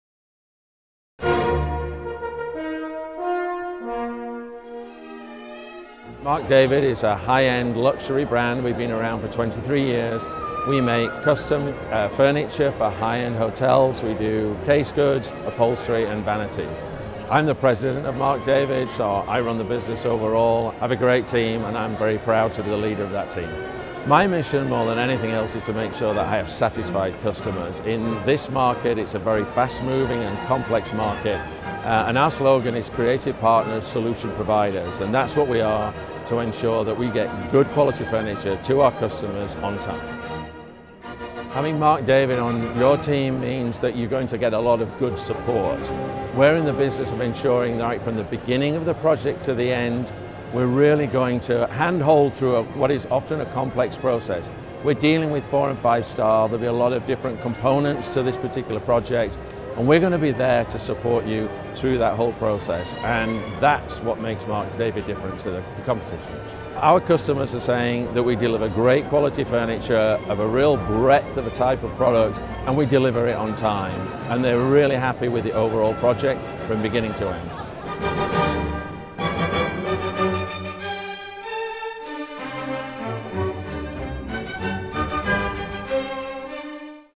Roger Wilson - MARK DAVID
Mark David
Fine Custom Furnishings
Roger Wilson - President

For over 20 years Mark David has satisfied growing worldwide demand for fine commercial contract furniture. Founded in 1990, the company has evolved into multi-national business with a strong presence in the U.S., Europe, Middle East and Asia. Customers are guaranteed a one-stop shopping experience with limitless custom products utilizing woods, metals, lacquers, paints, fabrics and rattans.

Product Categories include:
• Casegoods
• Seating
• Upholstery
• Vanities

Mark David has furnished some of the world's most prestigious hospitality destinations, luxurious hotels, high-end gaming facilities and award-wining resorts. Sales representatives around the globe understand the demands and complexities of challenging projects. Account executives work with customers on space limitations, electronics, technology, as well as guest needs and comforts to create the most innovative contract furnishings. In May 2008, Mark David joined the Kohler Co. family of brands, which includes Ann Sacks Tile & Stone, Baker Furniture, Kallista Plumbing and the McGuire Furniture Company.

KoniTV
Video Produced By:  Tri Cole